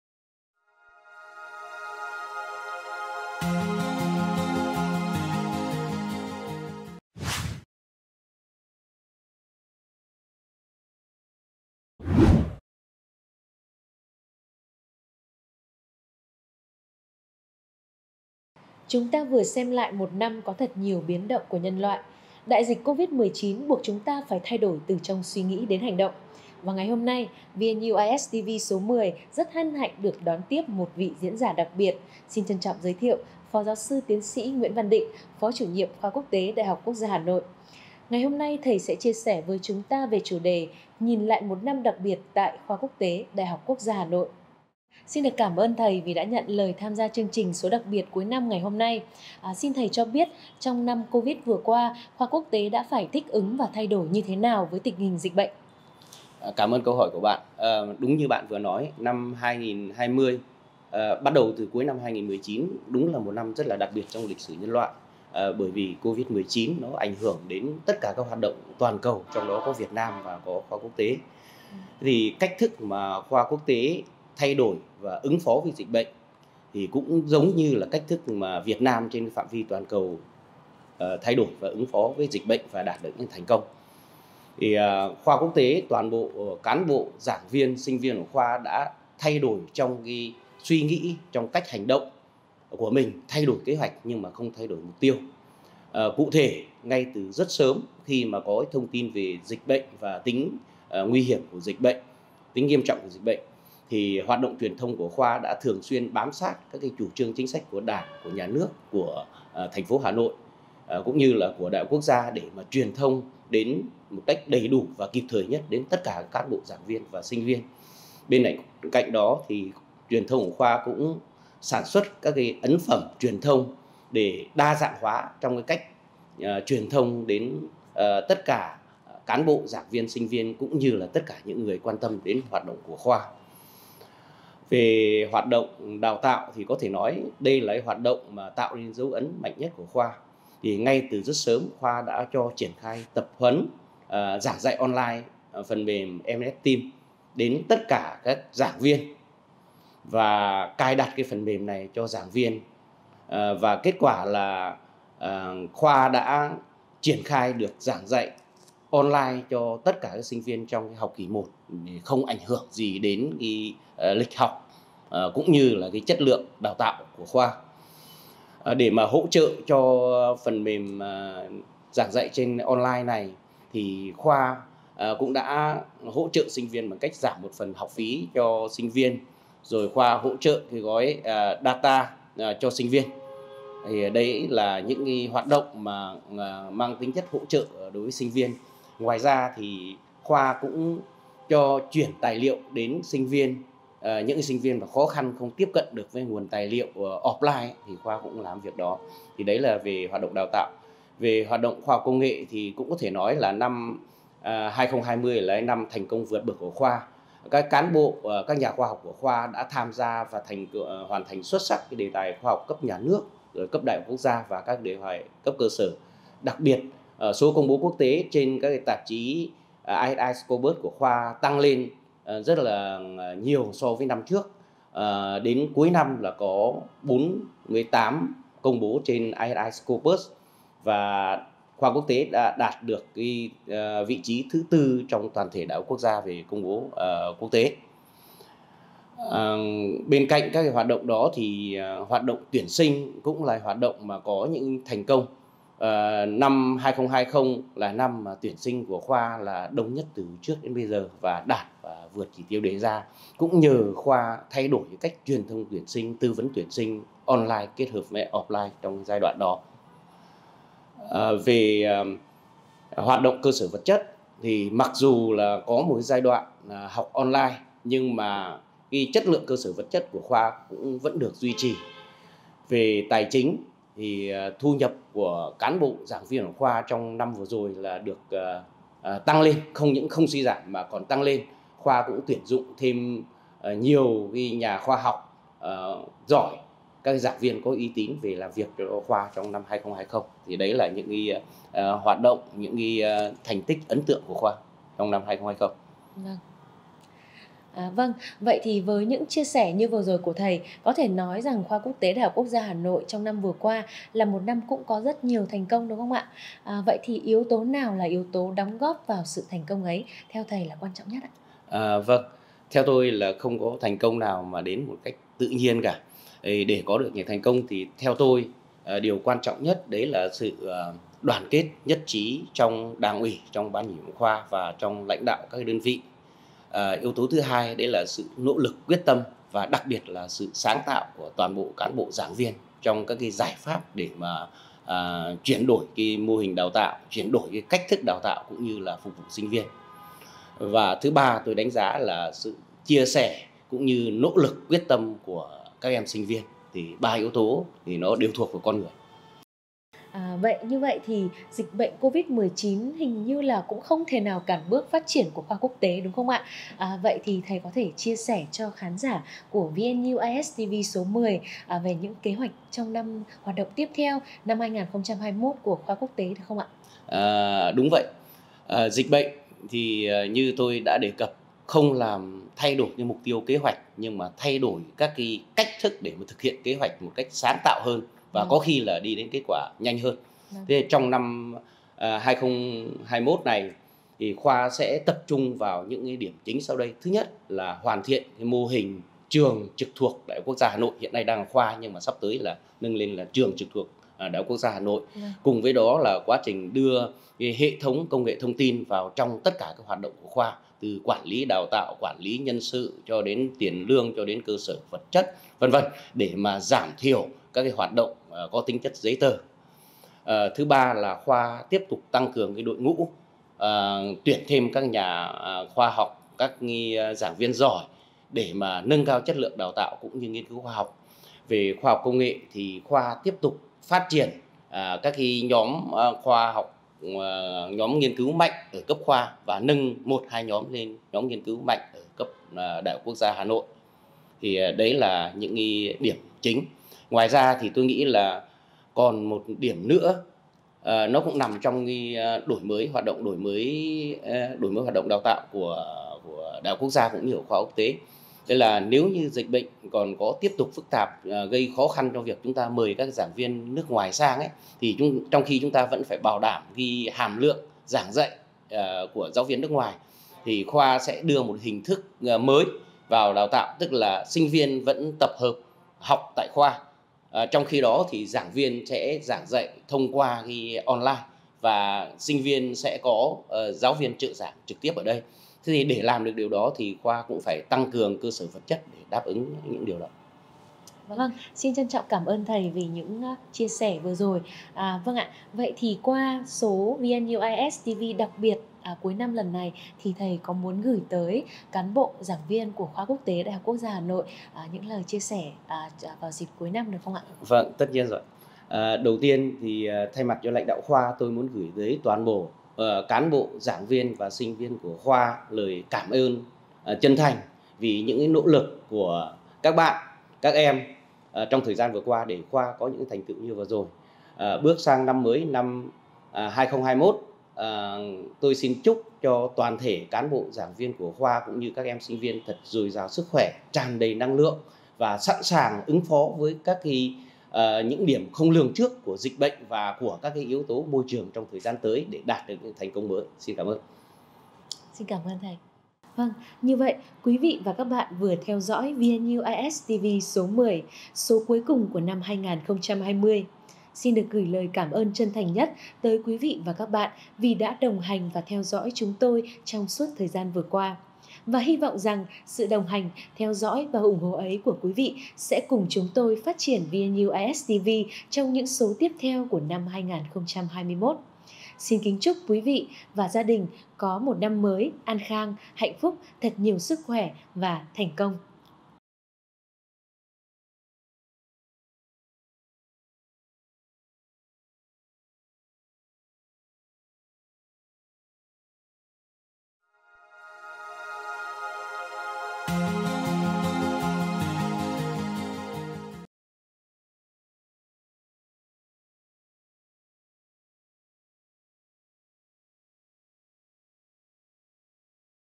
Looking back on an exceptional year of the International School, Vietnam National University, Hanoi
2020 has been an exceptional year in the history of mankind, due to the impacts of the Covid-19 pandemic to all human activities on a global scale, including Vietnam and the International School.

The way VNU-IS has changed and coped with the pandemic is similar to how Vietnam has managed to implement comprehensive policy and solution to achieve great success in the prevention and control of Covid-19.